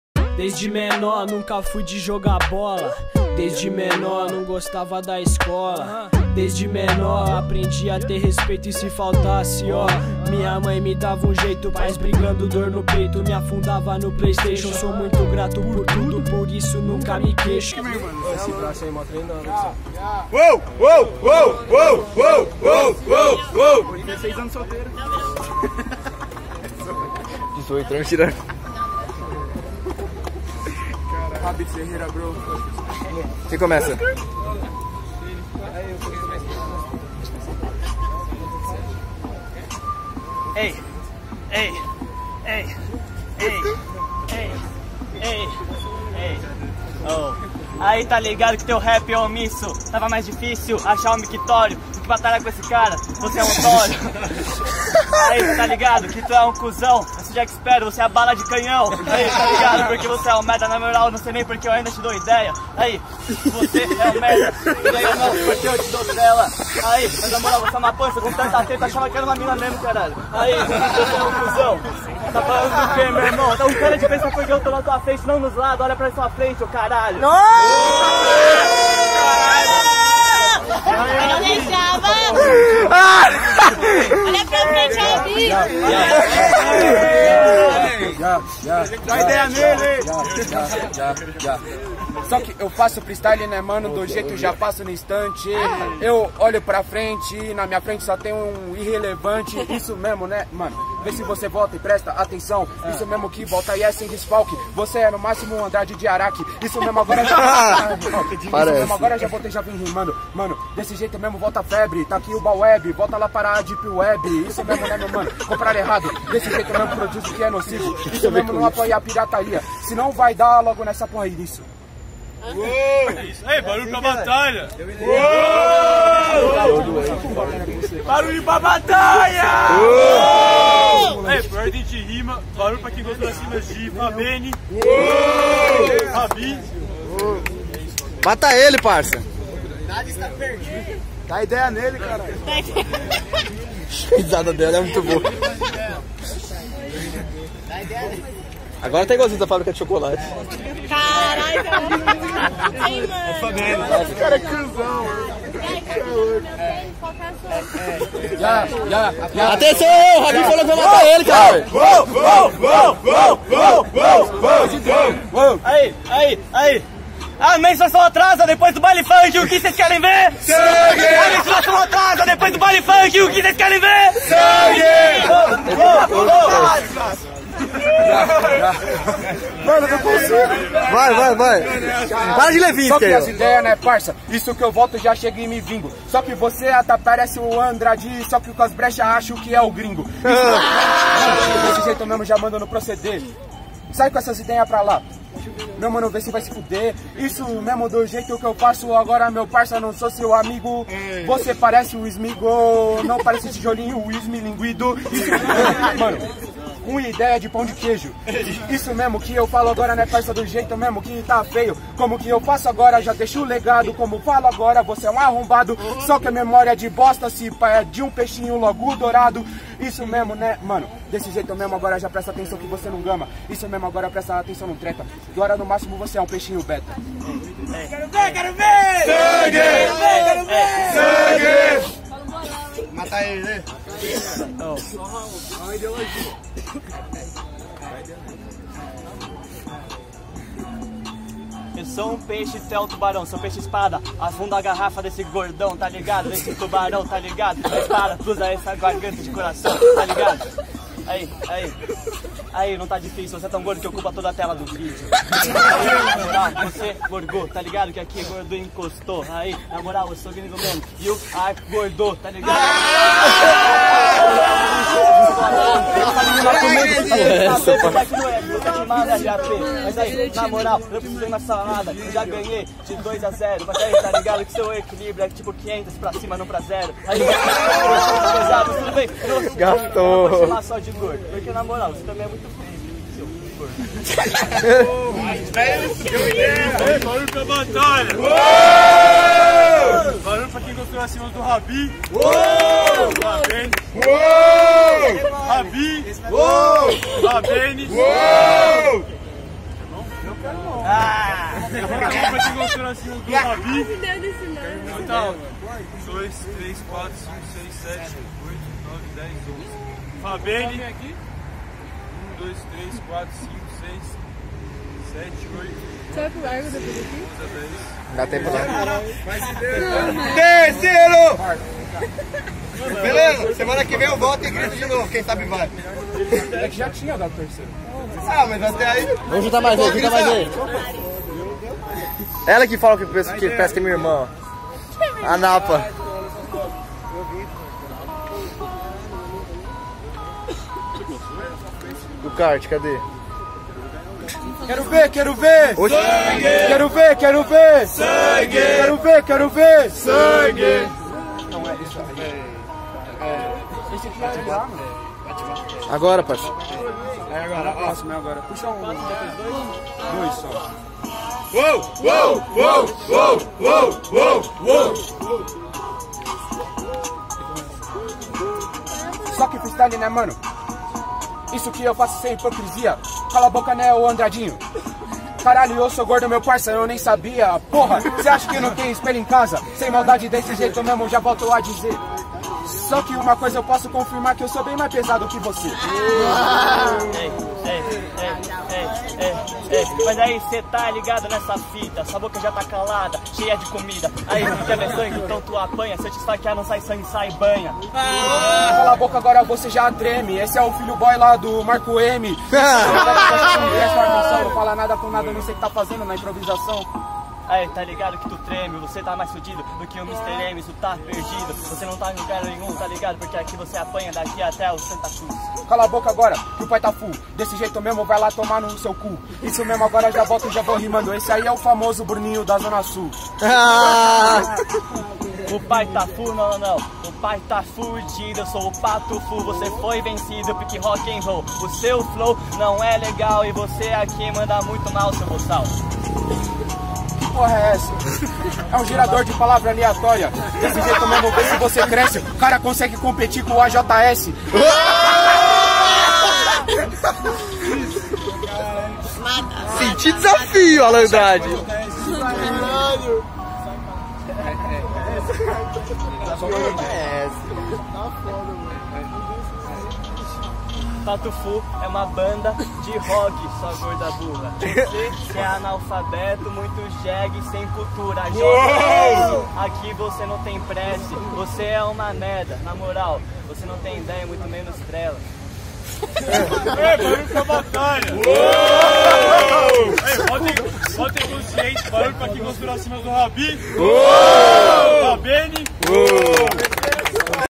Habi x Fabeni - 1FASE - 67° Batalha do Ana Rosa
Toda quarta-feira na Praça ao lado da saída do terminal de ônibus da estação Ana Rosa
Inicio da batalha: 19:00
Recomenda-se chegar antes para a entrega do nome e melhor organização.